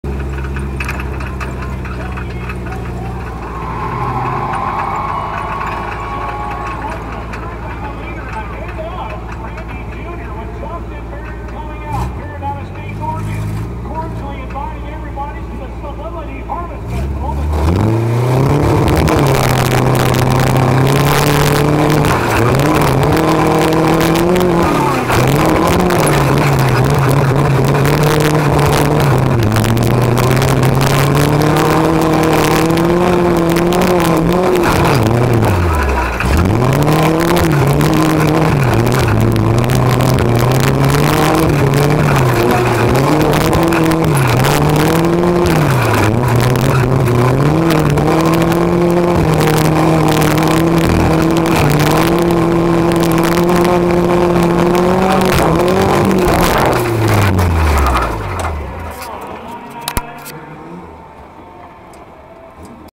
Grants Pass '11 Proarena Saturday round 1
Josephine County Fair proarena race 2011, saturday round 1. 5th place finish...